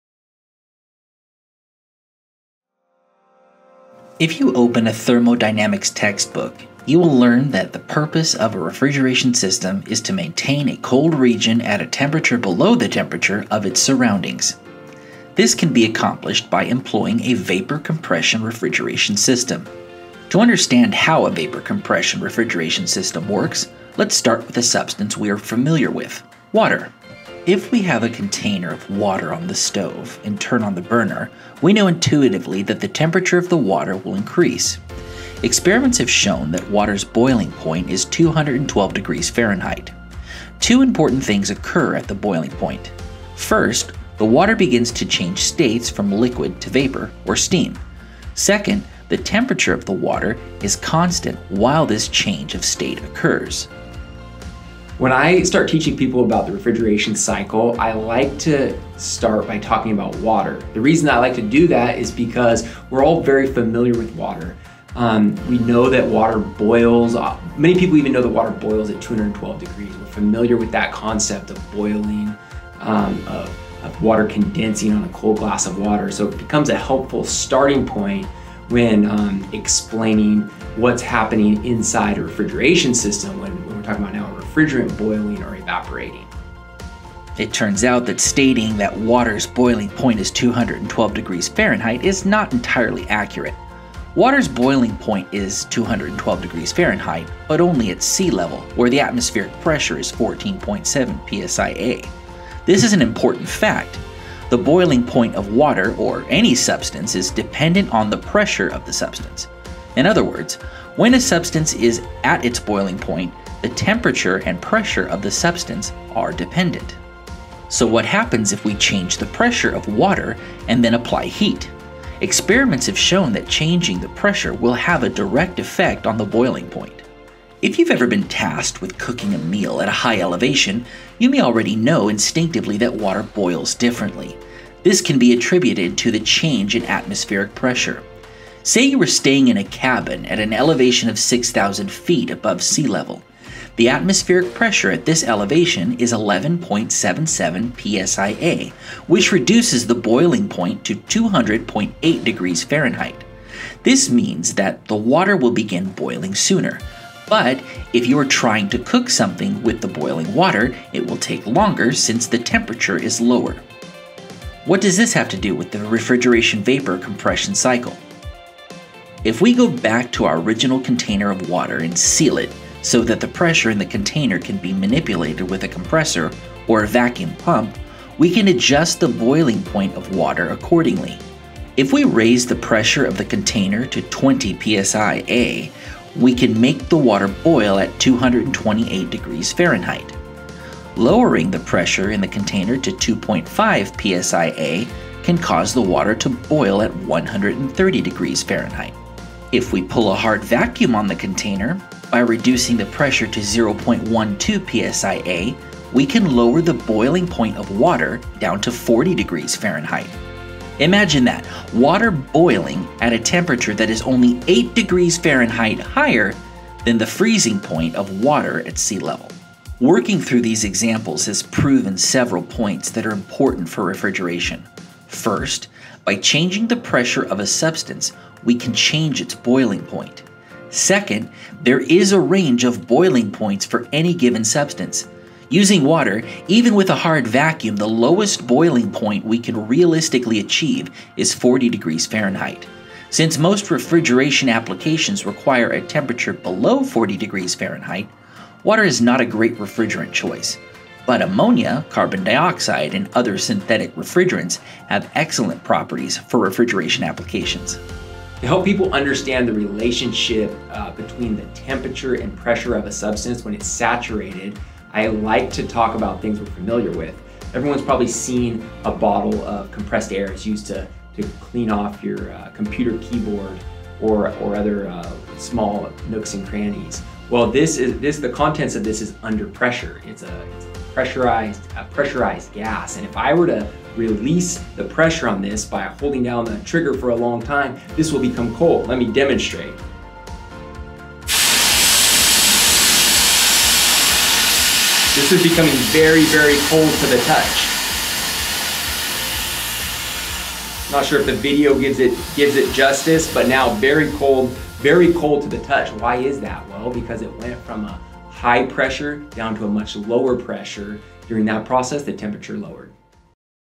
Changing the Boiling Point
Can we really make water boil at 40ºF? Watch this video to learn how you can manipulate the temperature at which a substance boils by changing its pressure.